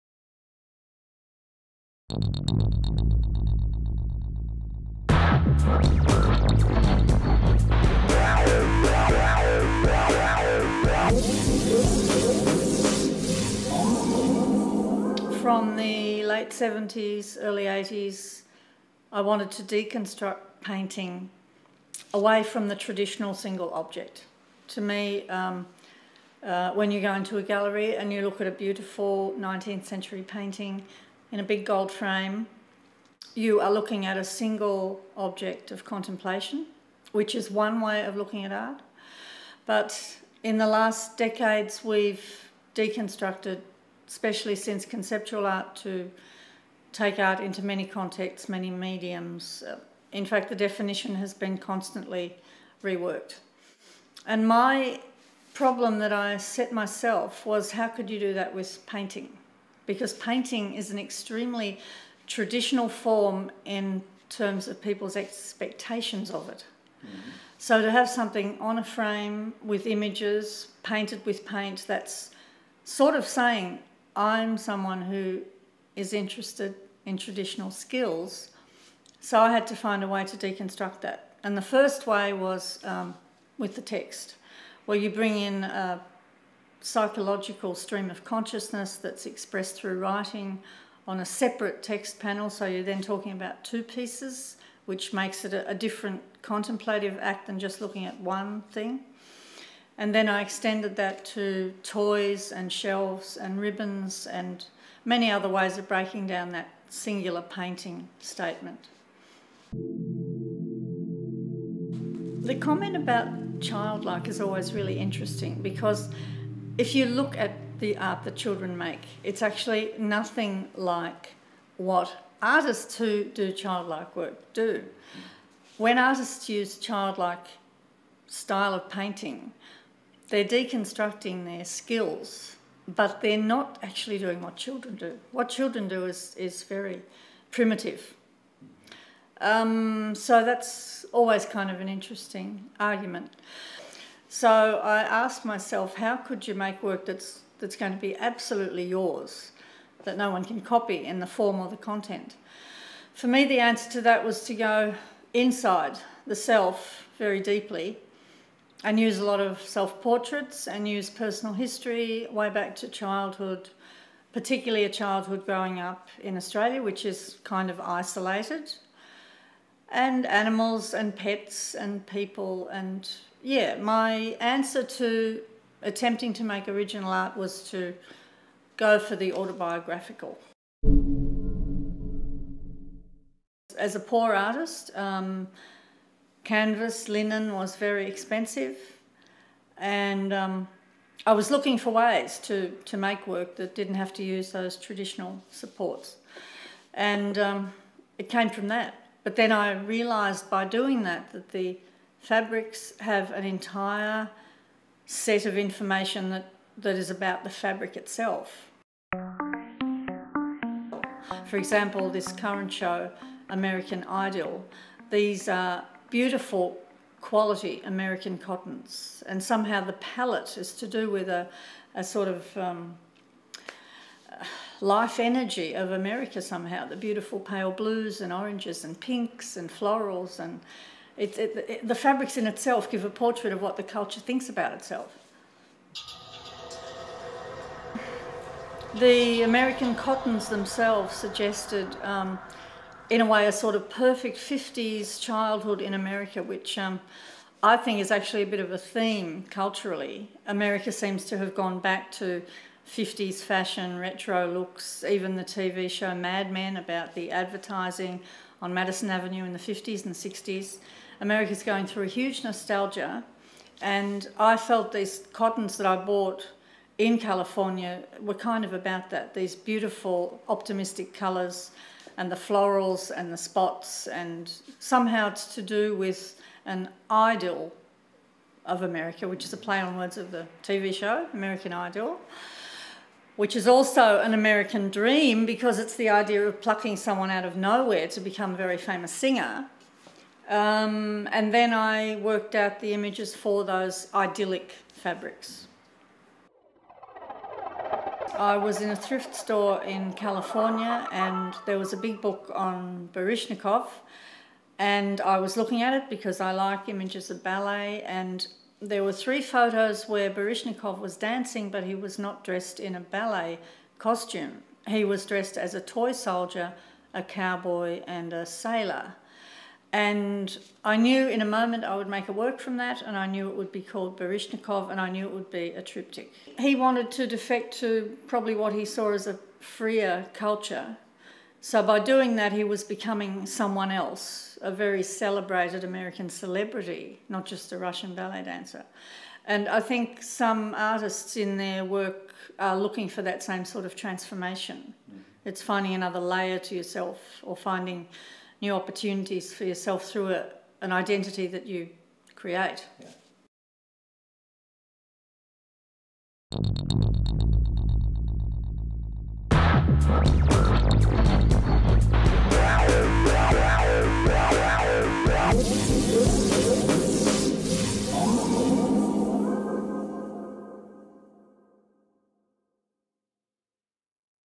exhibition . Jenny Watson | ENG
Presentation of the exhibition "American Idyll" of the Australian artist Jenny Watson organised by the art gallery Transit.

Let Jenny Watson bring you into her universe. After this intriguing journey, you will never look to tissues and America the same again.

Een film van | Un film de | A film by

Sophie Spillemaeckers
Ludovic Schweitzer